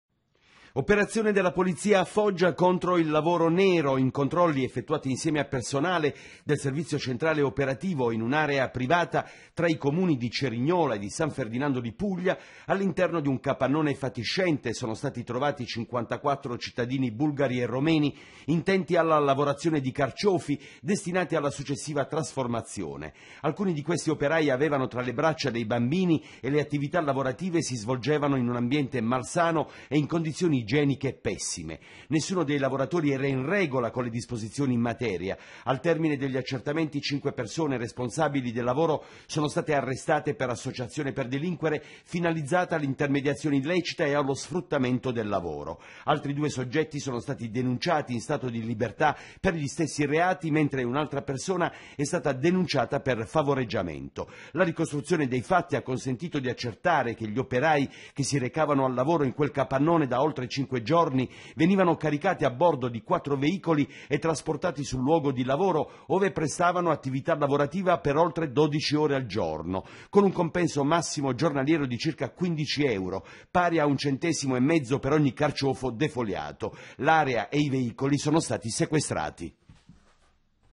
TG 28.03.14 15 euro al giorno per defoliare carciofi, "liberati" 54 nuovi schiavi nel Foggiano
Operazione della polizia a Foggia contro il lavoro nero. In controlli effettuati insieme a personale del servizio centrale operativo, in un'area privata, tra i Comuni di Cerignola e di san Ferdinando di Puglia.